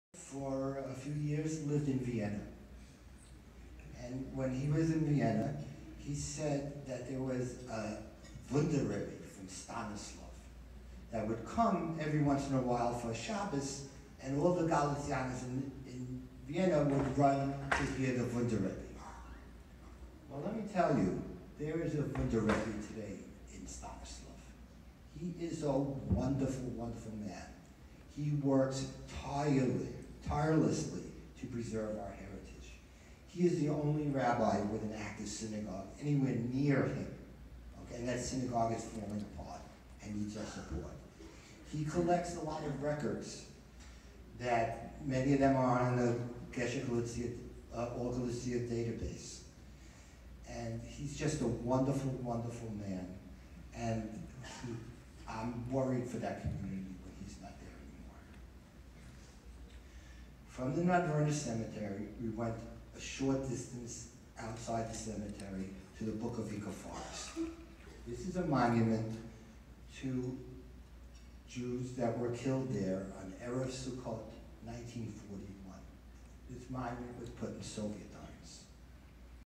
Gesher Galicia IAJGS Luncheon 2019: Speaking about Rabbi Moyshe Kolesnik & Bukowina Forest Memorial
Dr. Steven S. Turner Speaking at Gesher Galicia Luncheon During IAJGS Cleveland 2019.

Dr. Turner led a group of descendants of the shtetl of Nadworna (now Nadvirna, Ukraine), to the town in 2018. Here he was speaking about Rabbi Moyshe Kolesnik of Ivano-Frankivsk and the memorial in the Bukowinka forest to the victims of the mass shooting in 1941